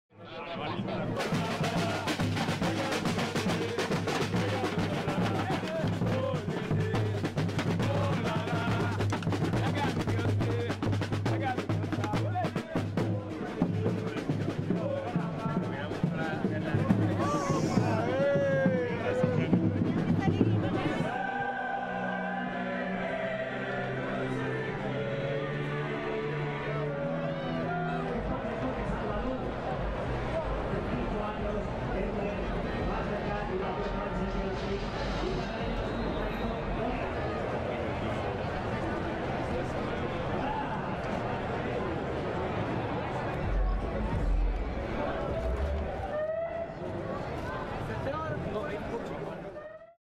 Champions League trophy shown off in Madrid as final countdown begins
The Champions League trophy was paraded through the streets of Madrid on Thursday (May 30) ahead of Saturday's final between Tottenham Hotspur and Liverpool.